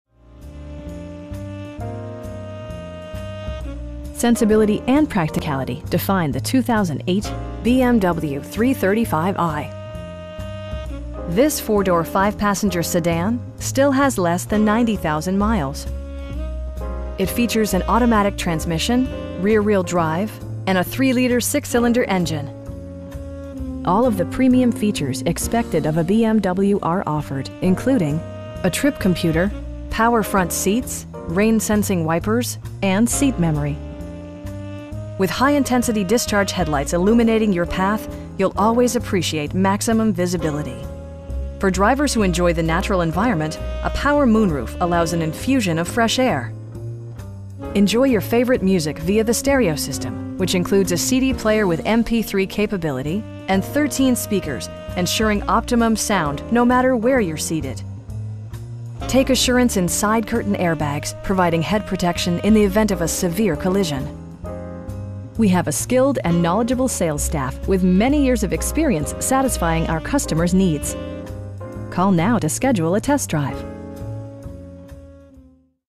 2008 BMW 335i in Portland, OR 97201
BMW Portland
2001 SW Jefferson St

Sensibility and practicality define the 2008 BMW 335i. This 4 door, 5 passenger sedan still has less than 90,000 miles! It features an automatic transmission, rear-wheel drive, and a 3 liter 6 cylinder engine. The engine breathes better thanks to a turbocharger, improving both performance and economy. All of the premium features expected of a BMW are offered, including: front and rear reading lights, 1-touch window functionality, a trip computer, power front seats, heated door mirrors, rain sensing wipers, and seat memory. With high intensity discharge headlights illuminating your path, you'll always appreciate maximum visibility. For drivers who enjoy the natural environment, a power moon roof allows an infusion of fresh air. Take assurance in side curtain airbags, providing head protection in the event of a severe collision. We have a skilled and knowledgeable sales staff with many years of experience satisfying our customers needs. We'd be happy to answer any questions that you may have.